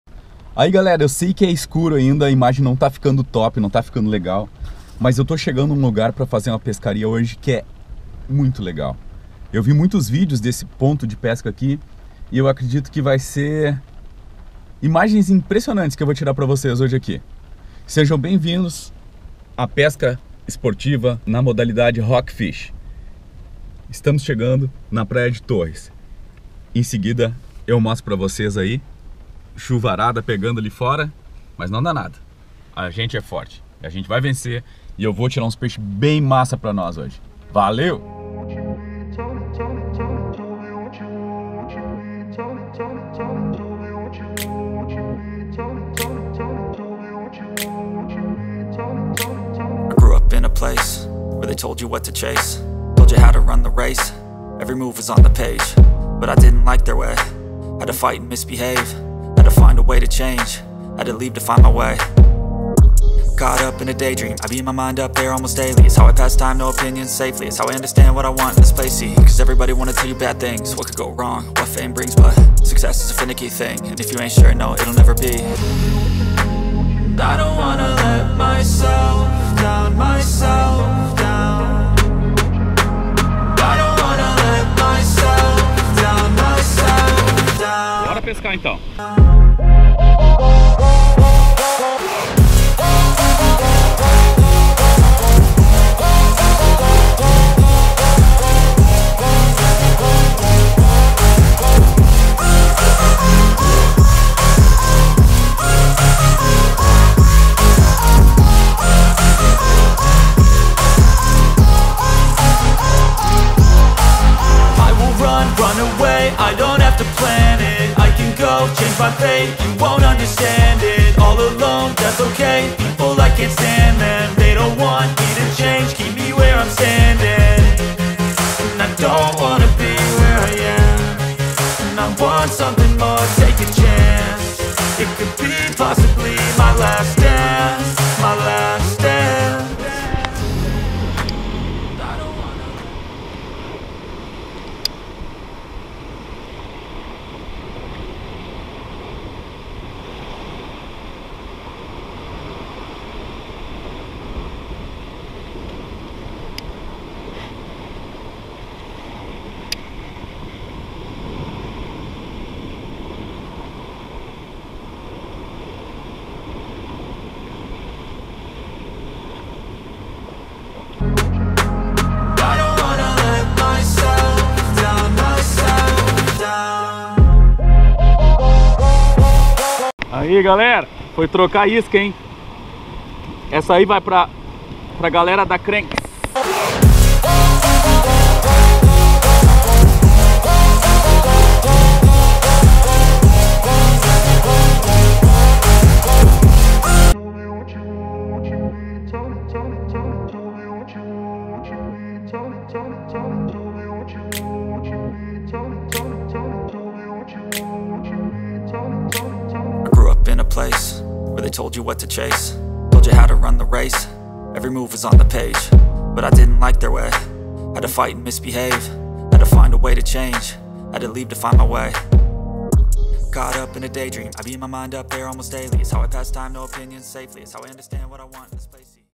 FURNAS DE TORRES [só o começo]
1º vídeo com edição , para deixar os pescadores com aquela vontade de quero mais .
E muito em breve terá uma série de vídeos compactos para entender as dificuldades que nesta modalidade de pesca.

*iscas kranx--
*PROFISHING IMPORTS -- (JEAN CARLO)👉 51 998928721